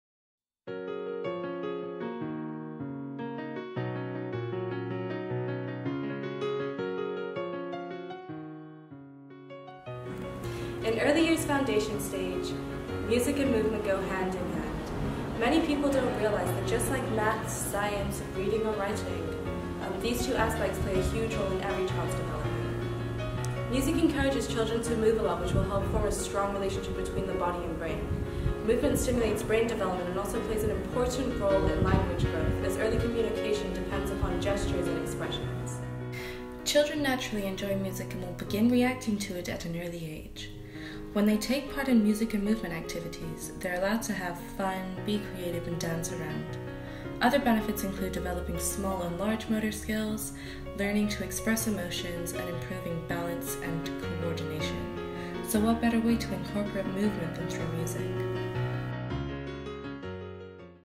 Importance of music & movement for children
In early years foundation stage, music and movement go hand in hand. Many people don't realize that just like math science, reading and writing, these two aspects play huge role in every child's development.